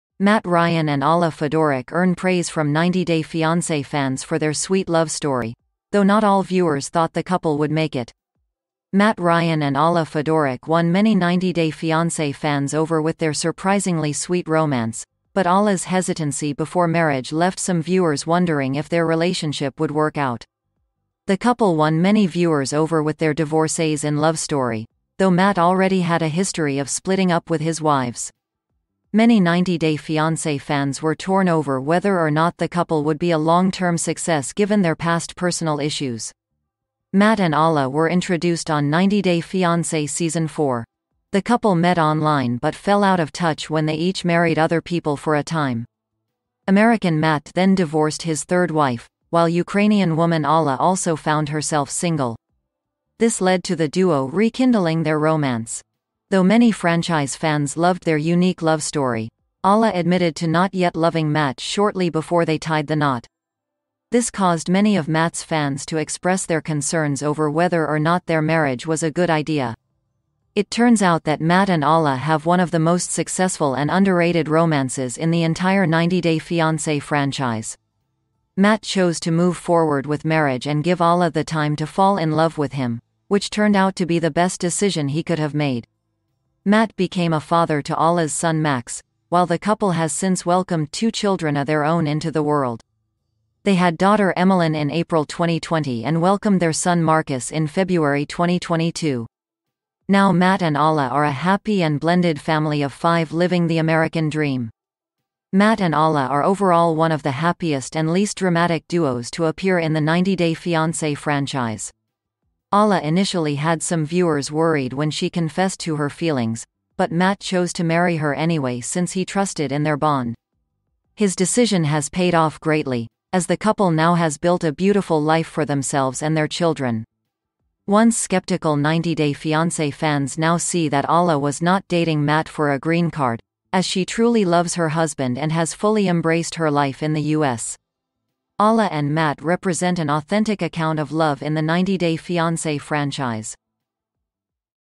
90 Day Fiancé: Are Matt Ryan & Alla Fedoruk Still Together as of 2022? Check Updates
Matt Ryan and Alla Fedoruk earn praise from 90 Day Fiancé fans for their sweet love story, though not all viewers thought the couple would make it.